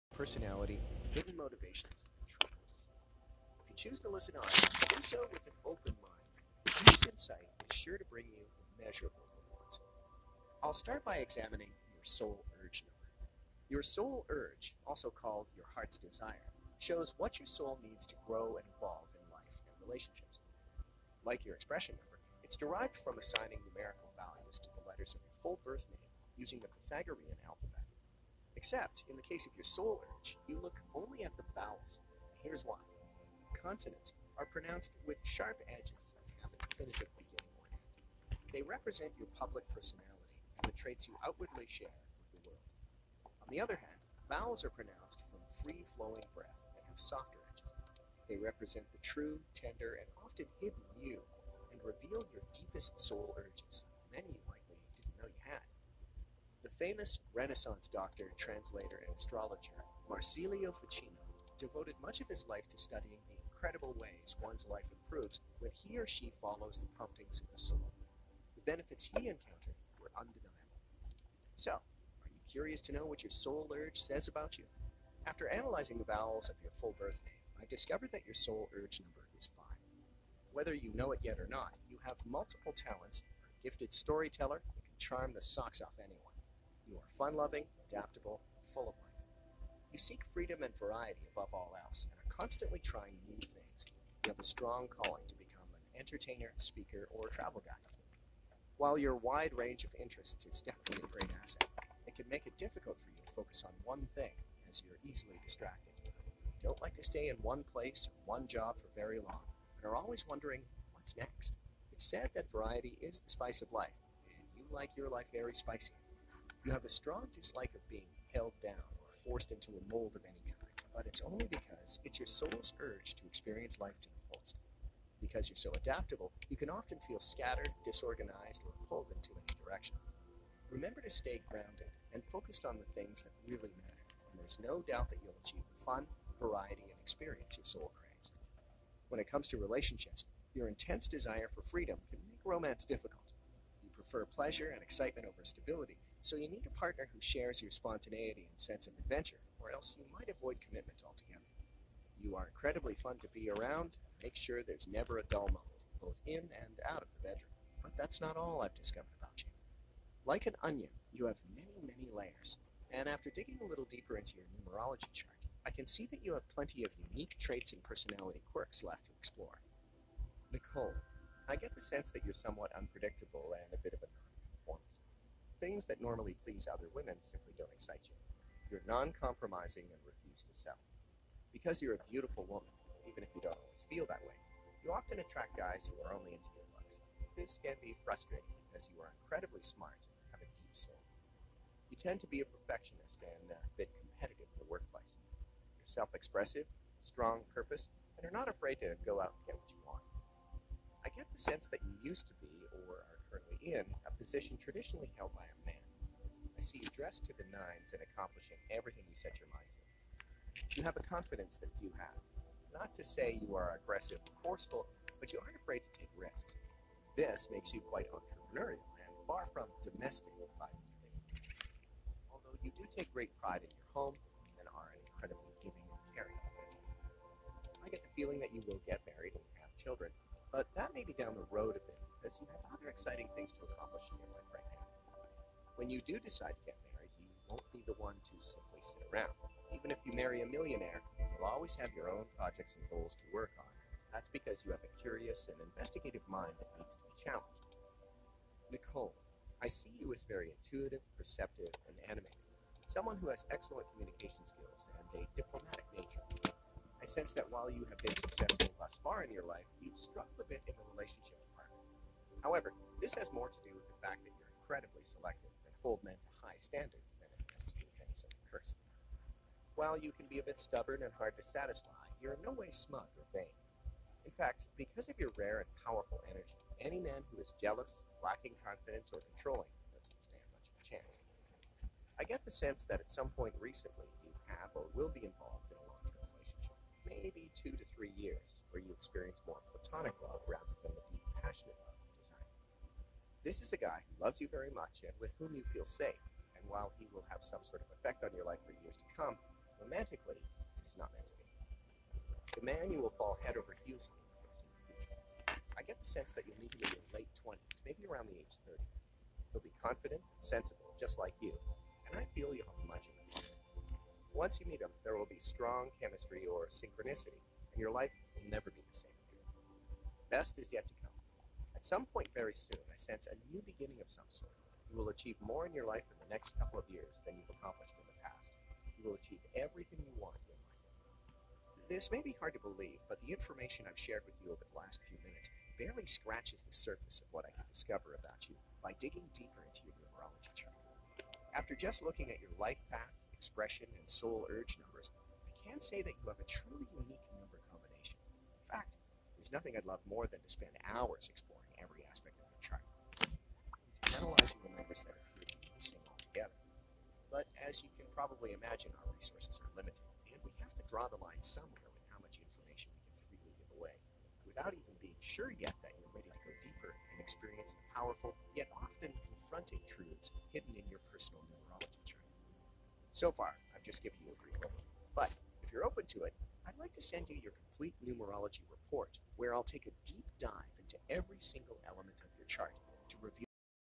Numerology
I did a numerology chart today and felt this was pretty interesting as it's my first chart I've done of this kind. What do you think? I found it fascinating.